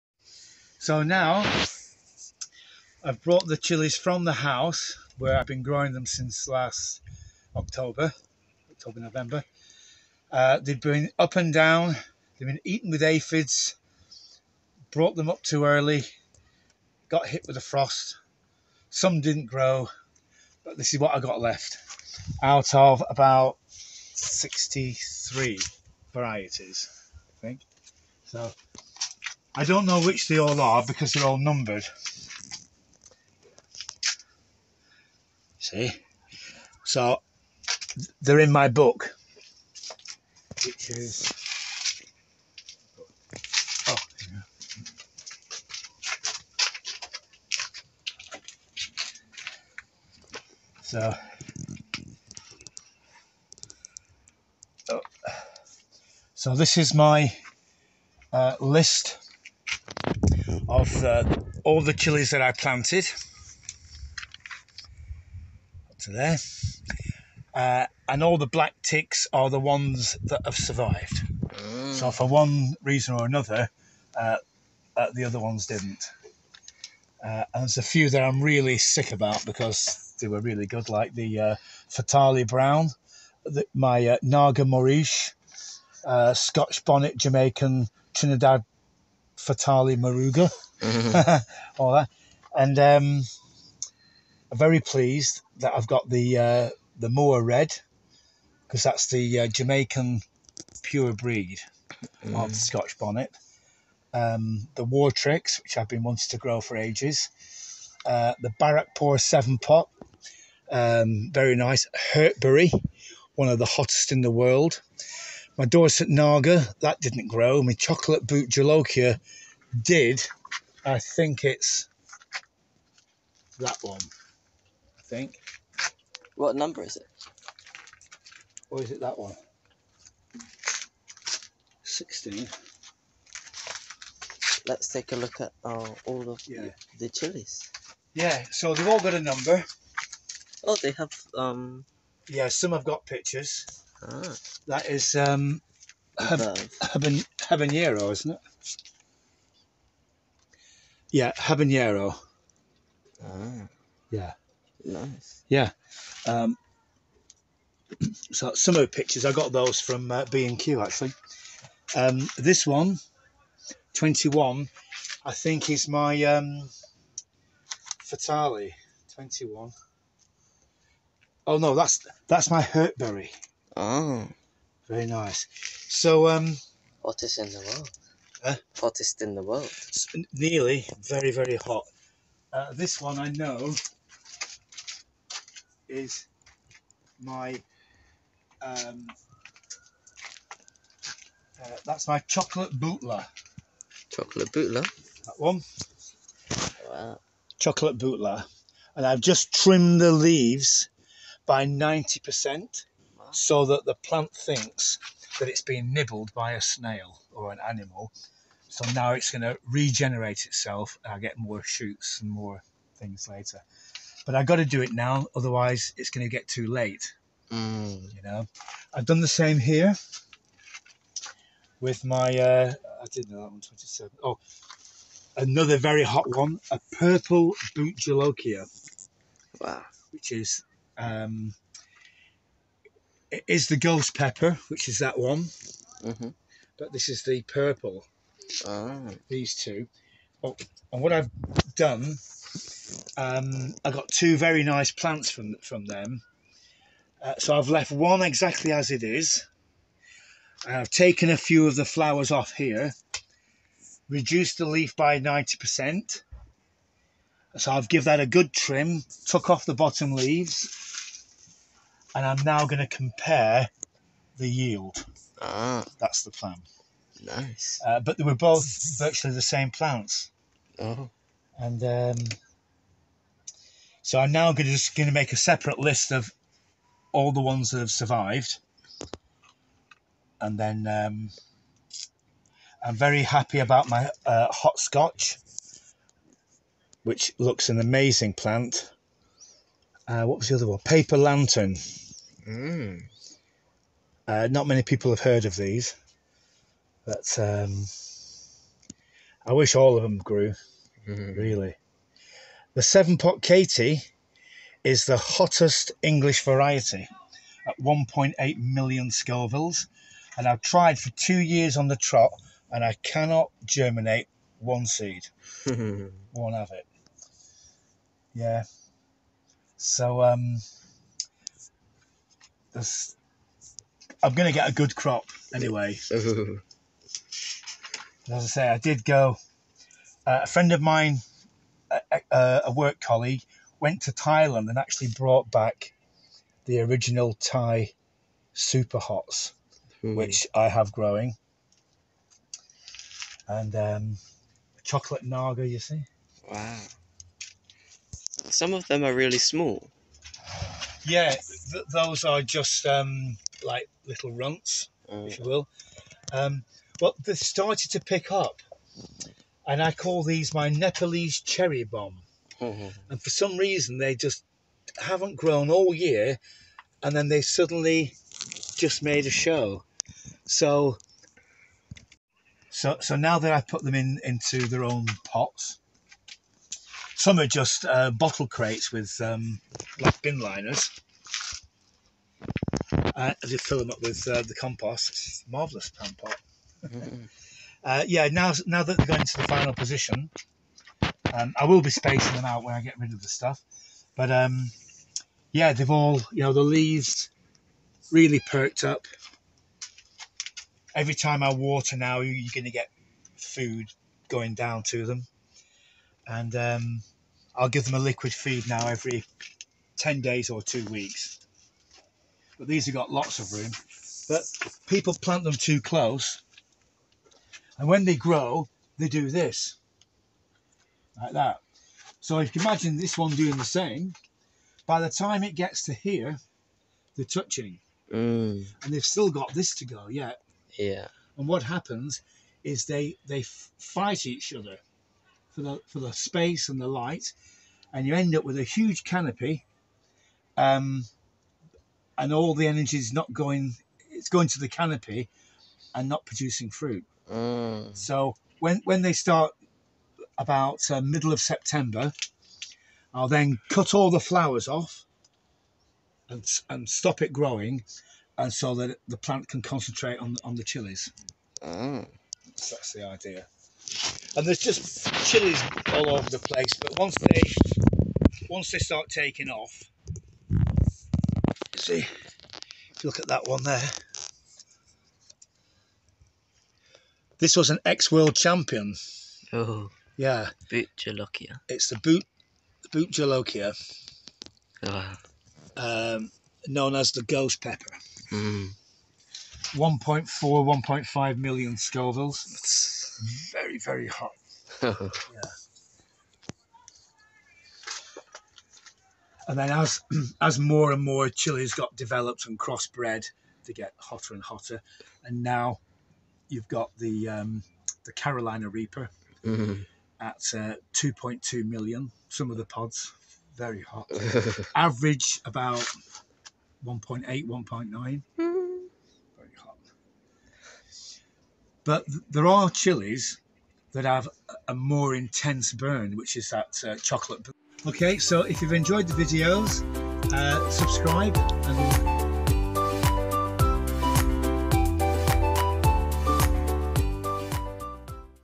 How To Grow World's HOTTEST CHILLIES (Pt. 1)
Join us on How To Grow World's HOTTEST CHILLIES

Edited by Oliver
@Oliver Green

Today Me, Mama, Papa have the best TIME EVER! It's SUMMER & we enjoyed ourselves in the fresh air and in the sun and have lots of fun looking at our blossoms, fruit trees and veggies.

The Best Full Comprehensive Way How To Grow World's HOTTEST CHILLIES with New Videos All The Time! It is lots of fun and laughs :)